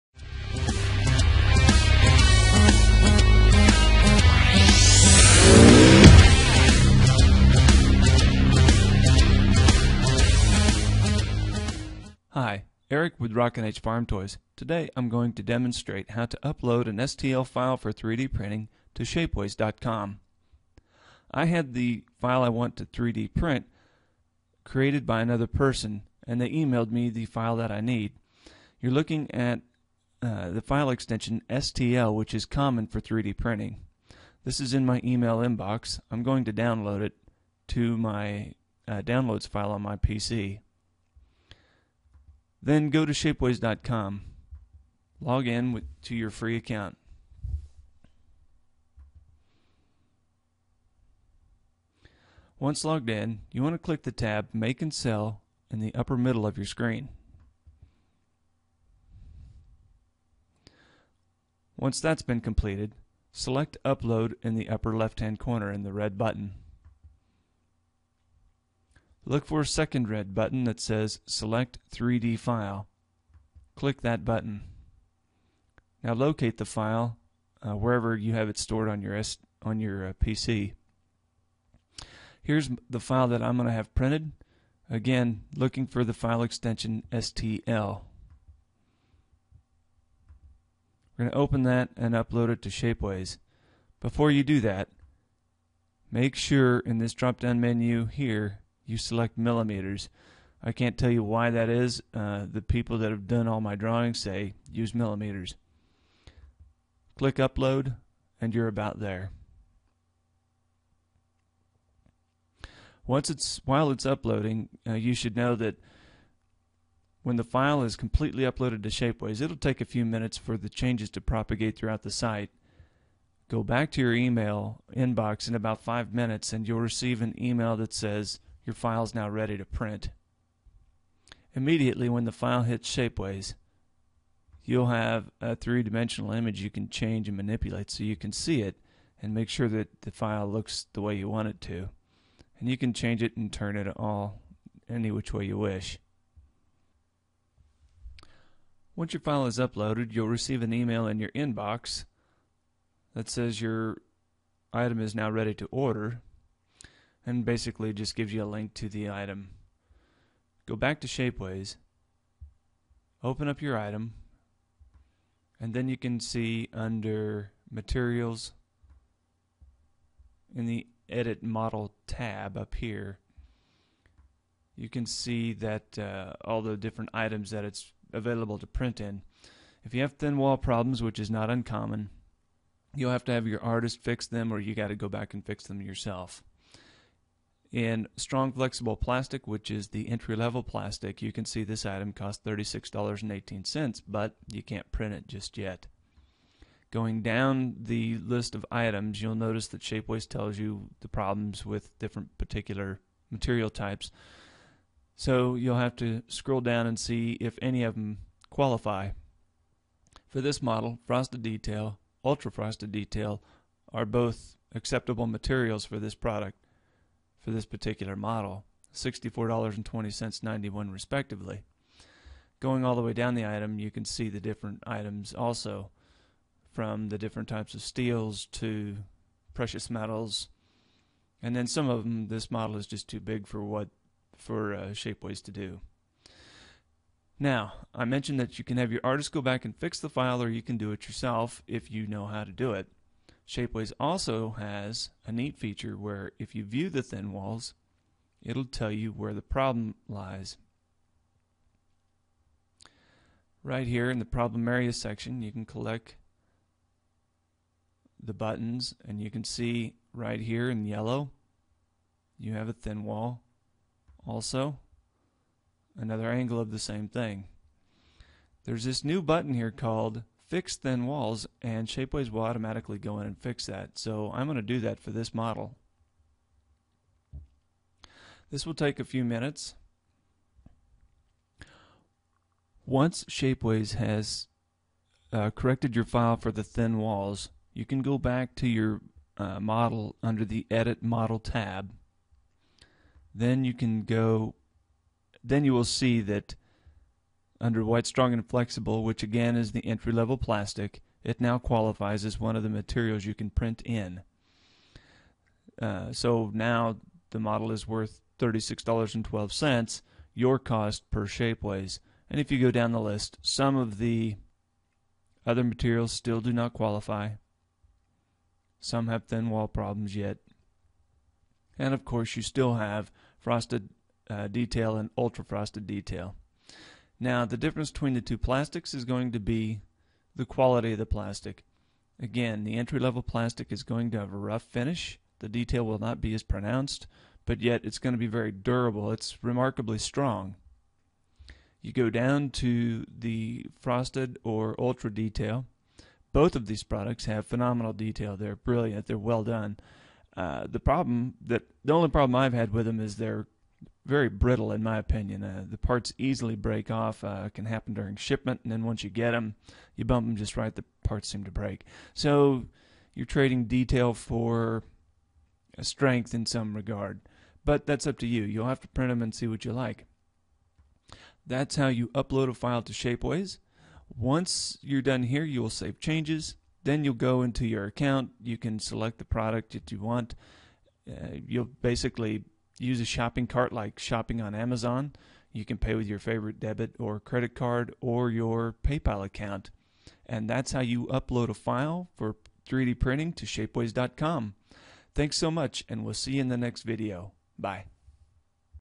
How to upload a file to Shapeways for 3D Printing
- I recently helped a friend create a 3D file for 3D printing. To help him learn how to upload the file and purchase a finished product I creates this short video you will find valuable.